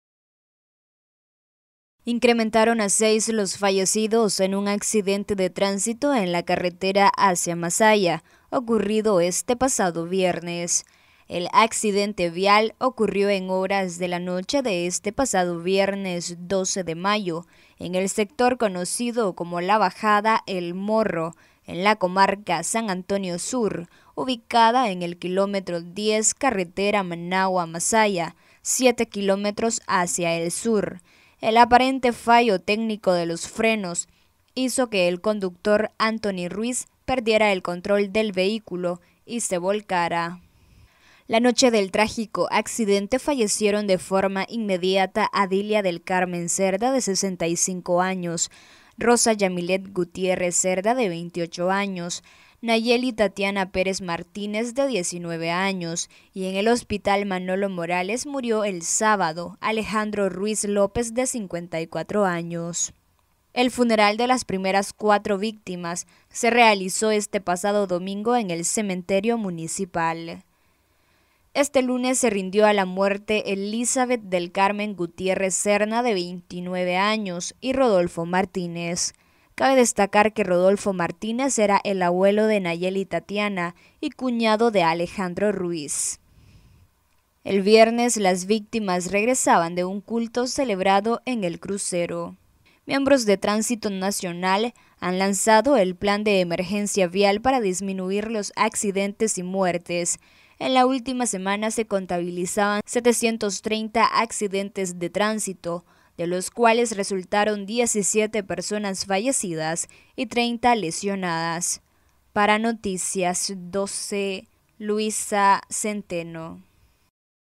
Noticias12Nicaragua Incrementan a 8 los fallecidos por el accidente vial en carretera hacía Masaya
Incrementan a 8 los fallecidos por el accidente vial en carretera hacía Masaya